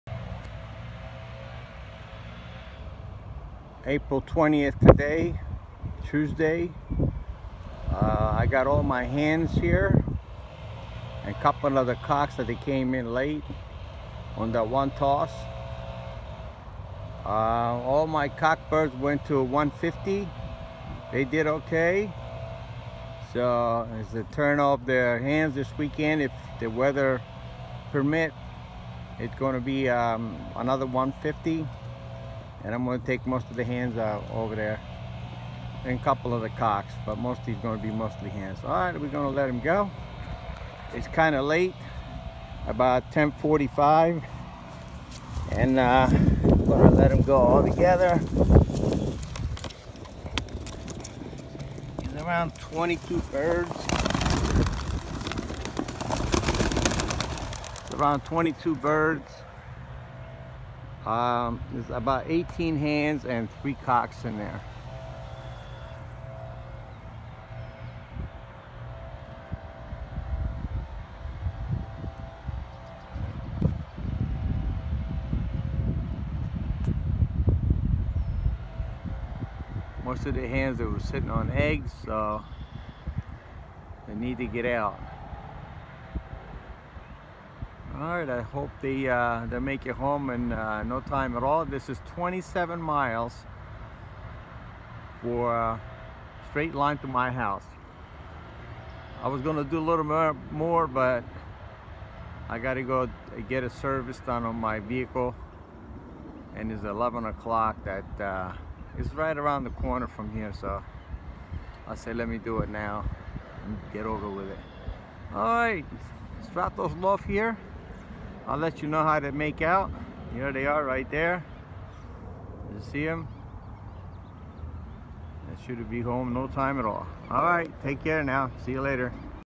Pigeon toss #16 ... 27 miles
I took all the hens and three cocks that come in late at my last toss .
I got the two birds back from the 150 mile race three days late but they all here 👍